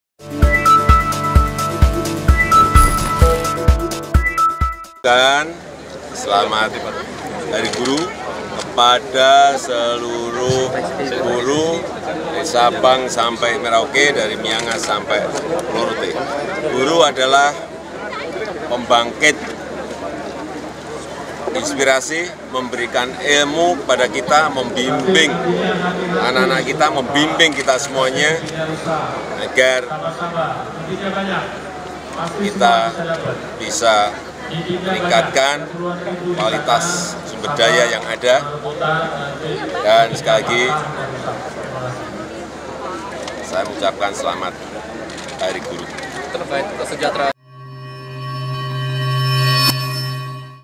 Jokowi Ucapkan Selamat Hari Guru
Indonesia kembali memperingati Hari Guru. Presiden Joko Widodo pun menyampaikan ucapan selamat dan menyebutkan bahwa guru adalah pembangkit inspirasi.

“Selamat Hari Guru kepada seluruh guru dari Sabang sampai Merauke, dari Miangas sampai Pulau Rote. Guru adalah pembangkit inspirasi,” ucap Presiden di Taman Wisata Alam Punti Kayu, Kota Palembang, Provinsi Selatan.

Selain itu, guru juga telah memberikan bimbingan untuk meningkatkan kualitas sumber daya manusia.

“Membimbing anak-anak kita, membimbing kita semua agar kita bisa meningkatkan kualitas sumber daya yang ada,” kata Presiden.